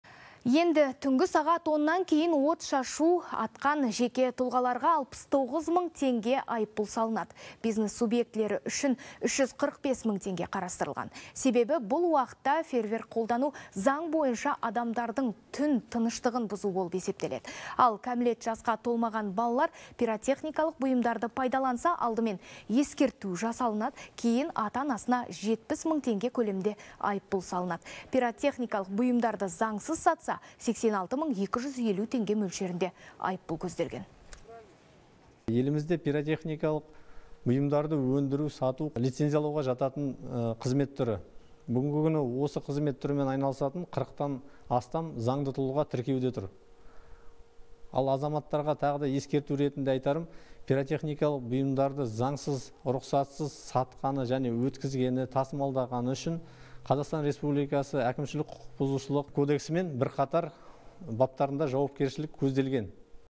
Отшашу атқандар 69 мың теңге айыппұл арқалайды
Енді түнгі сағат оннан кейін отшашу атқан жеке тұлғаларға 69 мың теңге айыппұл салынады. Бизнес субъектілері үшін 345 мың теңге қарастырылған. Себебі, бұл уақытта фейерверк қолдану заң бойынша адамдардың түн тыныштығын бұзу болып есептеледі. Ал кәмелет жасқа толмаған балалар пиротехникалық бұйымдарды пайдаланса алдымен ескерту жасалып, кейін ата-анасына 70 мың теңге көлемінде айыппұл салынады. Пиротехникалық бұйымдарды заңсыз сатса 86 мың 250 теңге мөлшерiнде айыппұл көзделген.